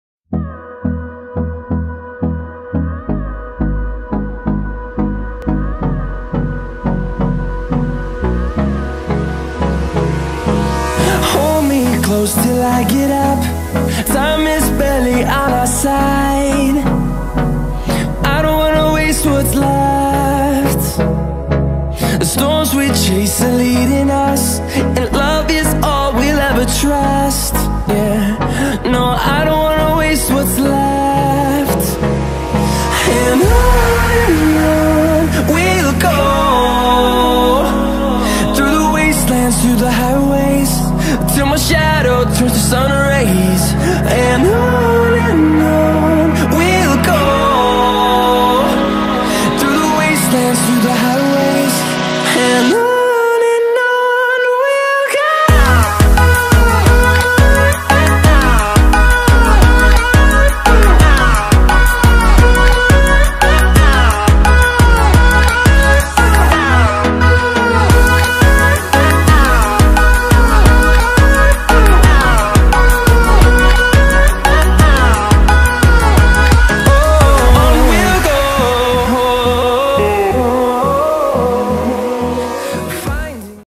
12 ความต่างระหว่าง "ตอนคบกัน VS ตอนเลิกกัน" ที่คู่รักหลายคนต้องเคยเป็น
โุไม่ทันกดหยุดภาพเอานะค่าบบบ